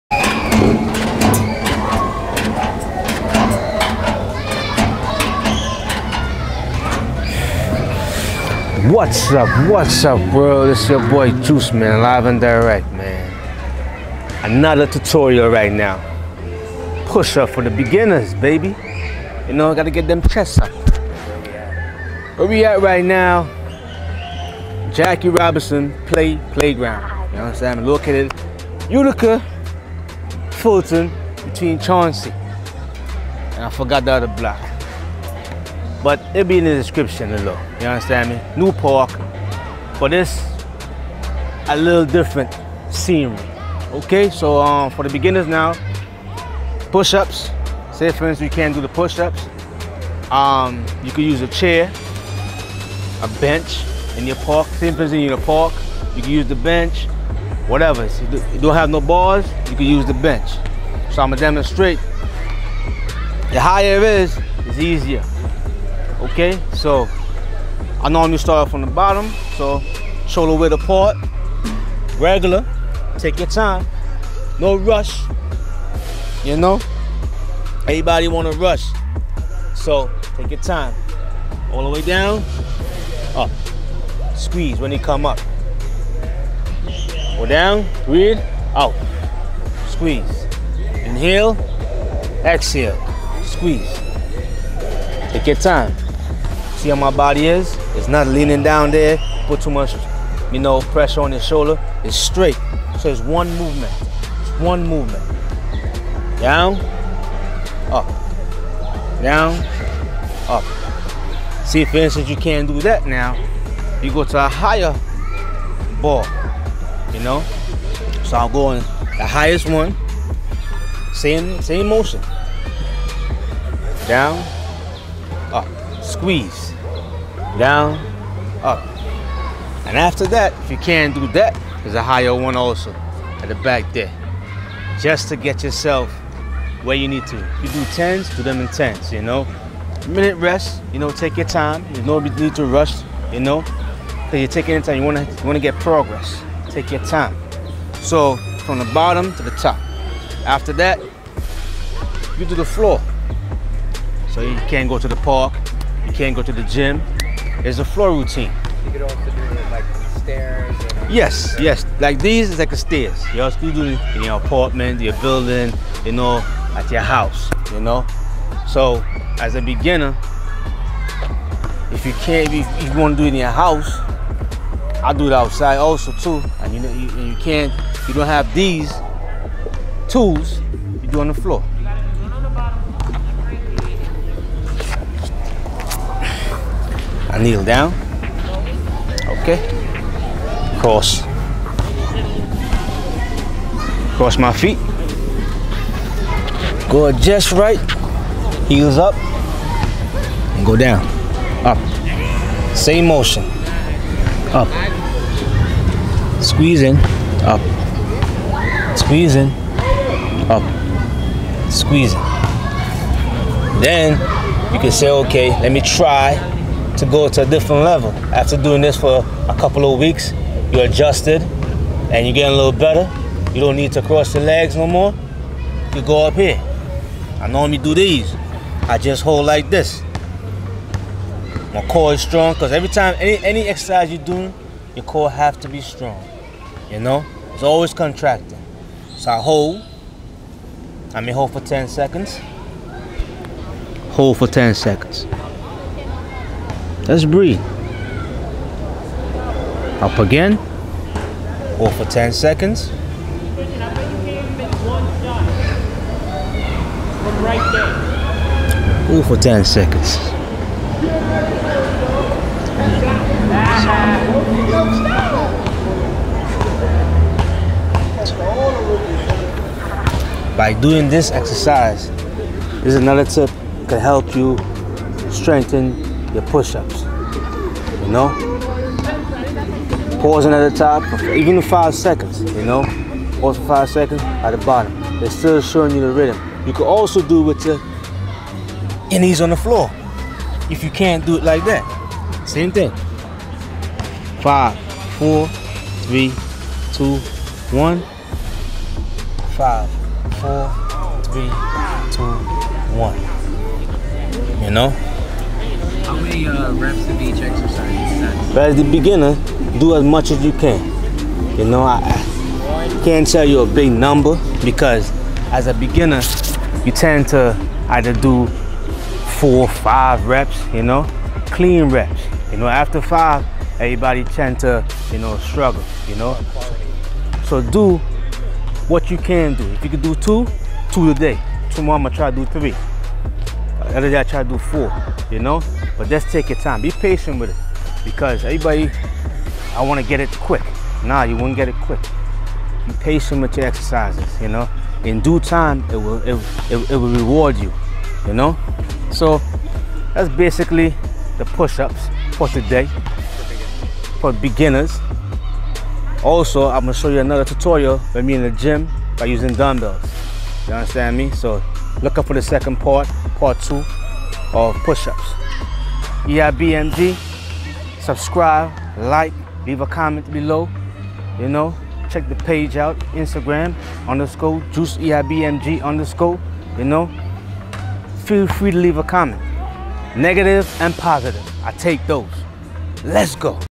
Pushups for the Beginner
The best way to get better at Pushups? This is about to sound crazy. The best way is by doing more pushups and strengthening your chest. I show you a couple exercises to strengthen your chest and the proper form when doing them.

A big shout out to my homie Ratul for supporting me. You can check out his stuff online and follow him on IG (@Ratul.Sharm)